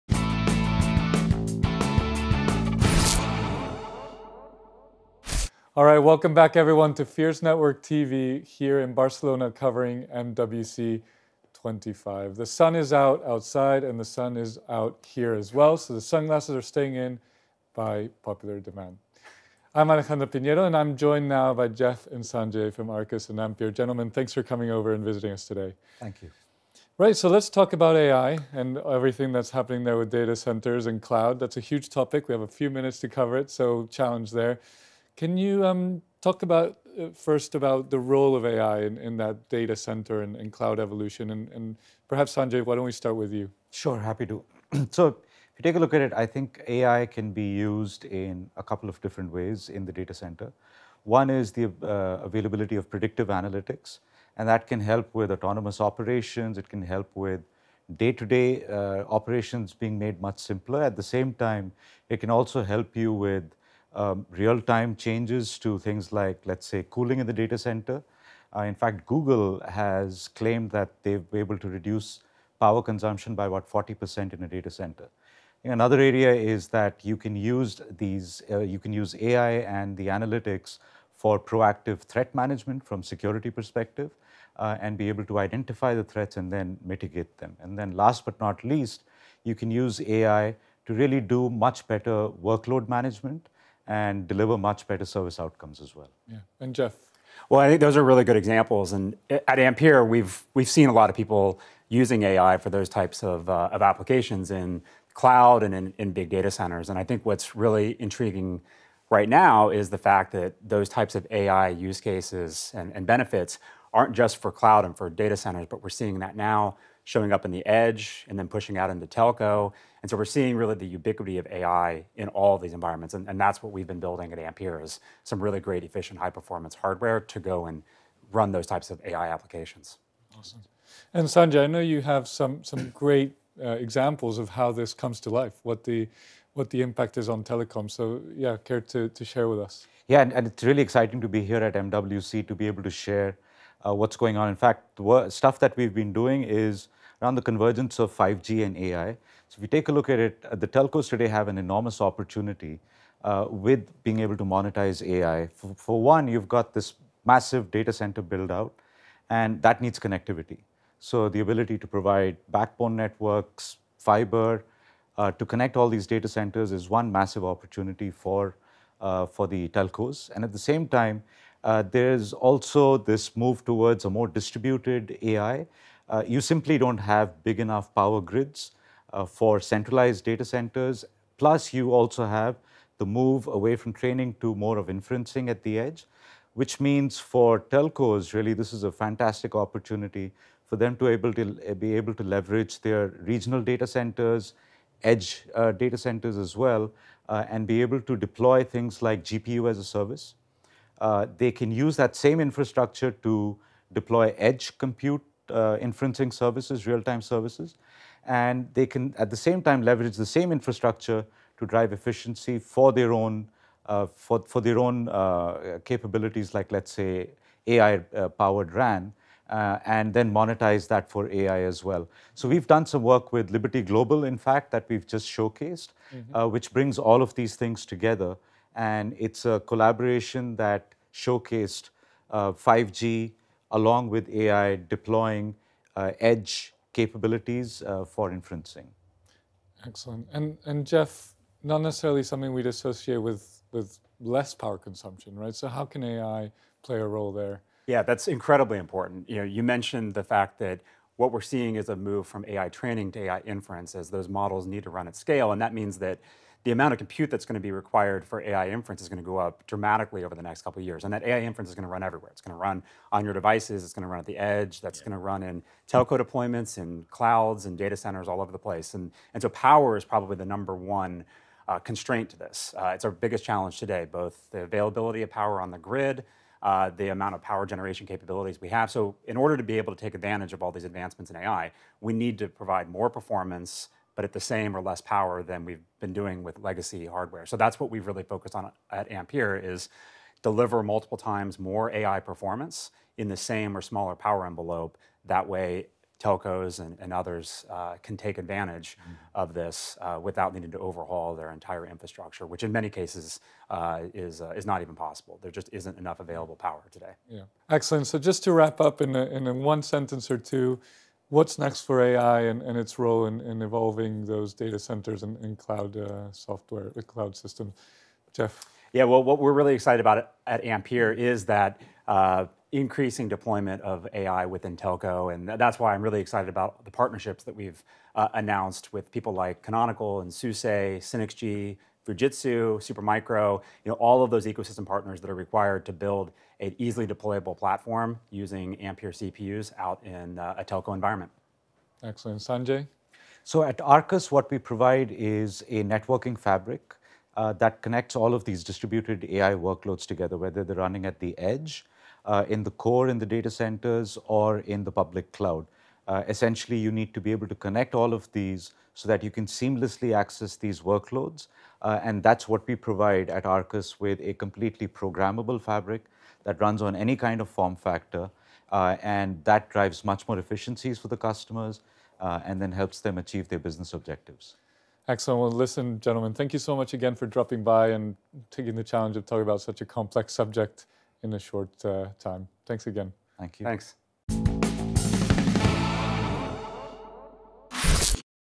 AI, Cloud, and Data Centers: Powering the Future of Telecom
Discover how AI is driving innovation in cloud and data center technologies, optimizing performance, enhancing energy efficiency, and shaping the future of telecom infrastructure.